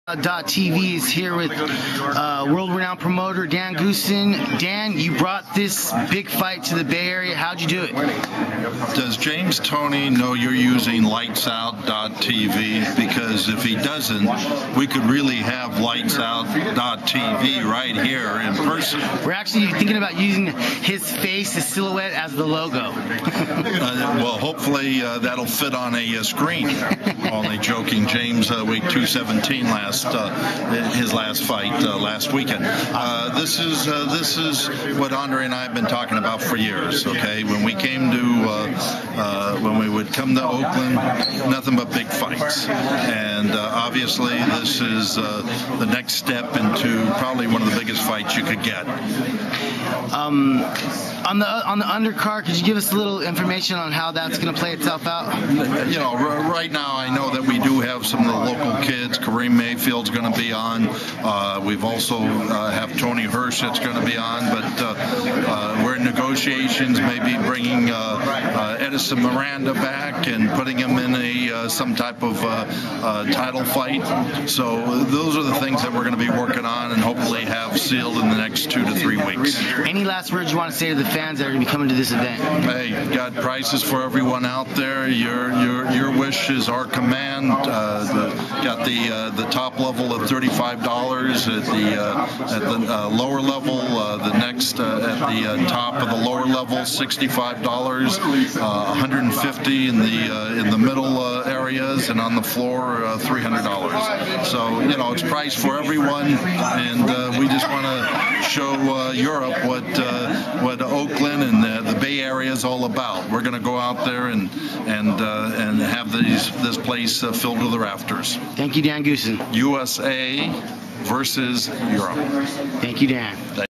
Dan Goosen Talks To LightsOut
Dan Goosen, promoter extraordinaire, gives LightsOut.tv his two cents.